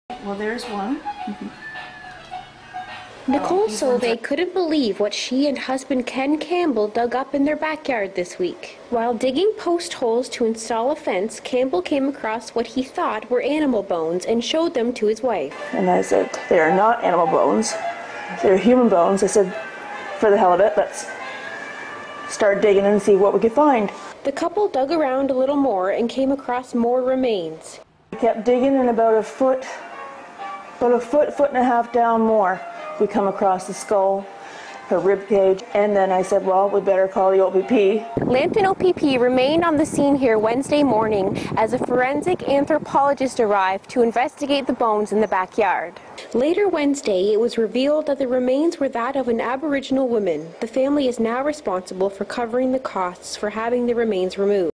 Couple Finds Ancient Skeleton In Backyard In Canada.
Couple Finds Ancient Skeleton In Backyard, May Have To Foot $5,000 Excavation Bill (VIDEO)A Canadian couple who unearthed the ancient skeleton of an aboriginal woman may also have to foot the thousands of dollars it cost to excavate the historical remains.

At first, Ontario resident Ken Campbell thought the bones he dug up while putting in a new fence earlier in June might be from an old animal skeleton. But Campbell's wife, Nicole Sauve, wasn't so sure, according to the Toronto Star.

"I said, 'They're not animal bones, Ken. Let's dig some more and see what we can find,'" Sauve told the outlet. And she was right.

The remains belonged to what researchers now believe was an aboriginal woman who died in the late 1500s or early 1600s around the age of 24, according to the Star.

Local law enforcement authorities cordoned off the scene while a forensic anthropologist and local coroner were brought in to investigate remains that have now been identified as a skull, femur and ribcage, according to The Observer.

Local Registrar of Cemeteries Mike D'Mello told the Blackburn News that the woman, who may be a distant relative of the nearby Aamjiwnaang First Nation, had been interred in a shallow "pit" grave, which is characteristic of aboriginal burials.

While thrilled with the discovery, Campbell and Sauve said they may now have to pay for the expensive excavation of the remains, nicknamed "Sephira" by Sauve. Ontario's 2002 Funeral, Burial and Cremation Services Act dictates that property owners are responsible for the costs of such an assessment, according to a follow-up by The Observer.

"I'm devastated by it," Sauve told The Observer of the revelation. "I did the right thing. I could have covered her back up."

The couple has appealed to local officials in an effort to avoid paying a bill estimated between $5,000 and $7,500, according to the Star.

In the meantime, the bones, which Sauve originally wanted to keep, have been re-interred at a special First Nation cemetery following a traditional ceremony performed by members of the First Nation.